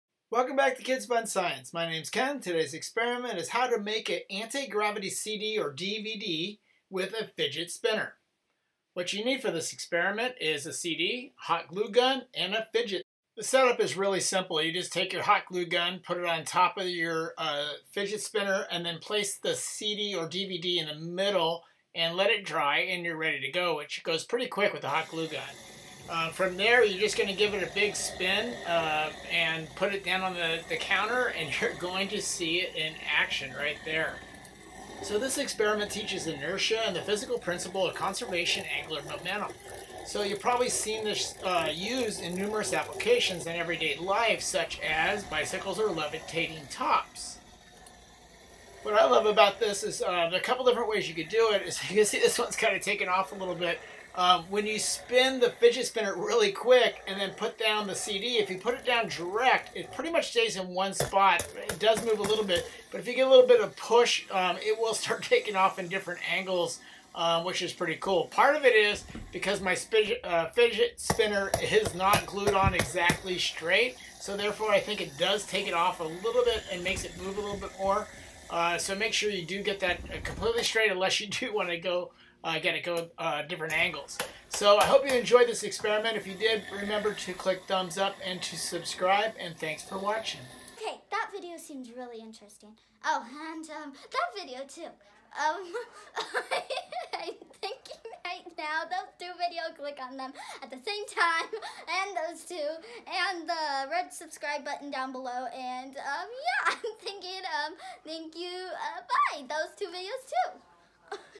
How to make a Anti Gravity CD Fidget spinner (GYROSCOPE CD with fidget spinner)
Anti Gravity CD Fidget spinner - GYROSCOPE CD with fidget spinner

Fidget spinner 5 pack

Chapters
0:00 Intro Kids Fun Science
0:09 What you need
0:13 Set up
0:25 Experiment
0:35 Science behind it

CD/DVD
Hot glue gun
Fidget spinner

This experiment teaches inertia and the physical principle of the conservation of angular momentum. You probably seen this used in numerous applications of everyday life such as in bicycles and levitating tops